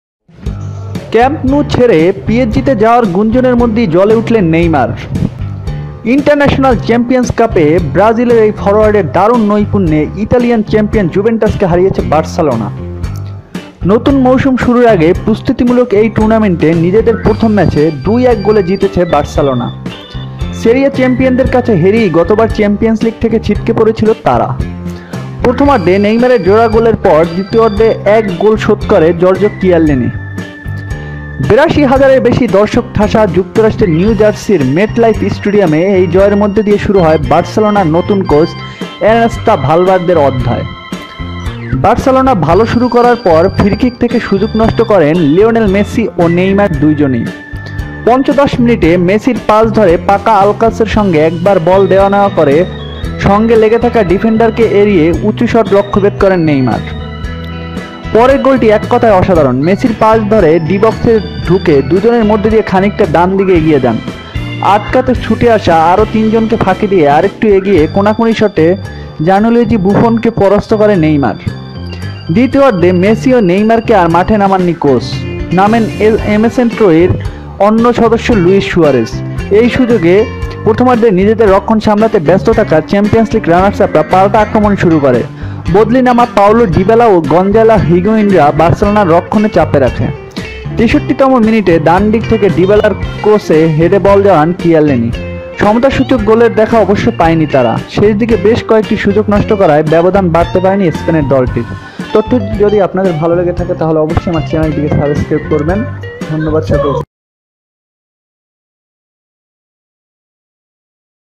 Barca, who lost the Siri A champions in the first match of the match , under the new coach
Niemair was shocked to leave the Camp Nou to go to PSG. Barcelona defeated Italian champion Uvantas in the best performance of the Brazilian forward for the International Champions Cup.

Barcelona won 2-1 in their first match in the preparatory start of the new season. They were knocked out of last Champions League by Serie A champions. In the first half, Giorgio Keyellini scored a goal in the second half after Neymar scored two goals.

Overcoming more than 82,000 visitors, Barcelona's new coach Ernesto Valverde has started this win at the Met Life Stadium in New Jersey.
After Barcelona started well, both Lionel Messi and Neymar missed the opportunity to freekick. In the 15th minute, Messi scored a ball with Pako Alcasar and took the ball away from the defender, who missed the defender.

The next goal is one extraordinary. Messi passes in the de-box and move on to the right in between the two. Neymar, after scandalizing three more people to stop him, defeated Jeanluigi Bufphan in a second-round corner shot at Conakuni.
Messi and Neymar in the second half; Another member of the MSN attacker, Luis Suarez, In this opportunity, the Champions League runners, who are busy managing their own defense in the first half, started a counter attack. Payroll Diabla-Gonzalo Higuainra increased the pressure on the preservation of Barcelona.

In the 63rd minute, from the right side, the head of the cross at Divala crosses the key in the net. They did not get the equivalent of equalizer goals. The Spanish team could not raise the gap due to the loss of several opportunities in the end.